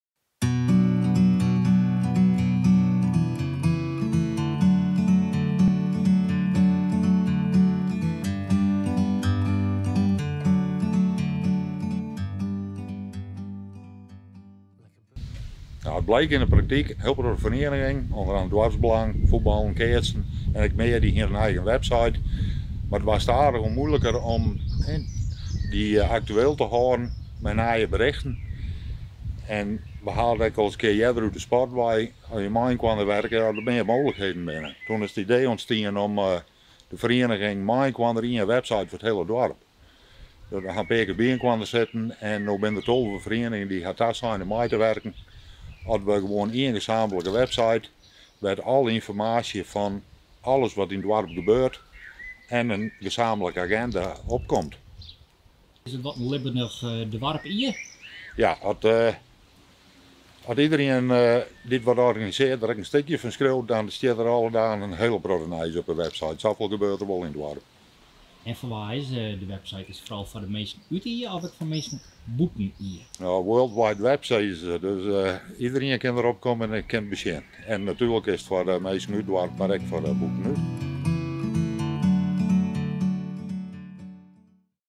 Dertien verenigingen uit Ee lanceren gezamenlijke website
Het vlasdorp Ee heeft een hechte gemeenschap en een bruisend verenigingsleven met veel activiteiten. Over een aantal maanden zijn deze zichtbaar op een nieuwe website, een samenwerkingsverband tussen dertien verenigingen uit het dorp.